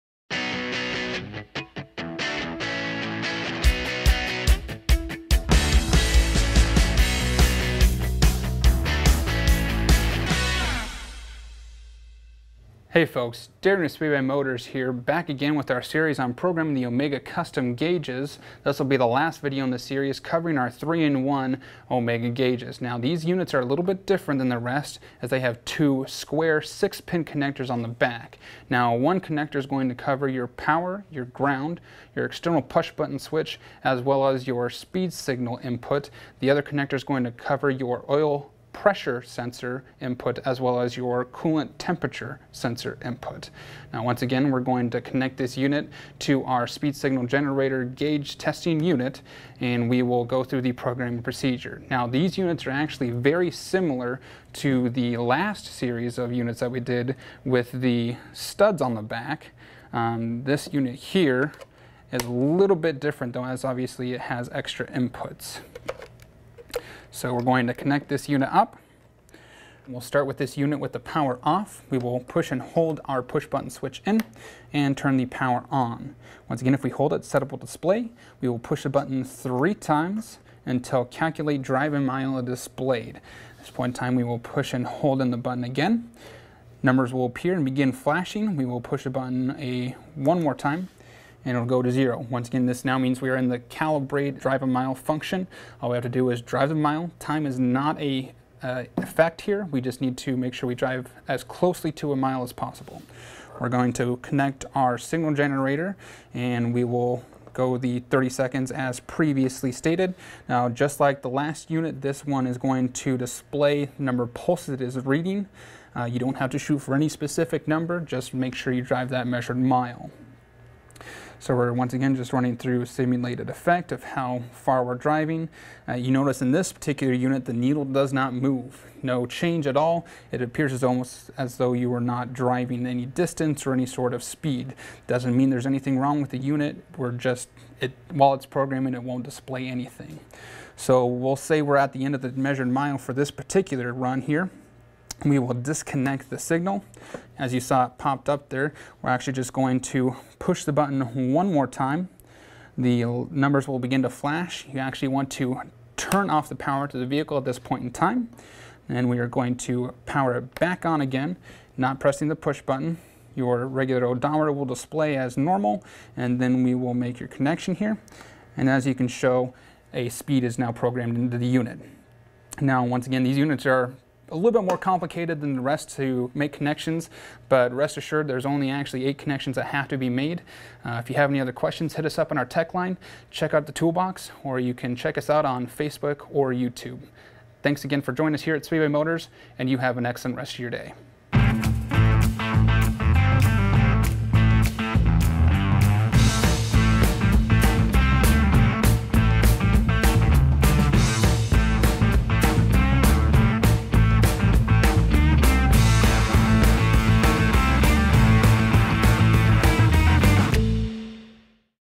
Tech Talk Omega Kustom 3-N-1 Gauge Set
In the third and final instructional video on the Omega Kustom Gauges, Darrian explains how to program the 3-N-1 omega gauge.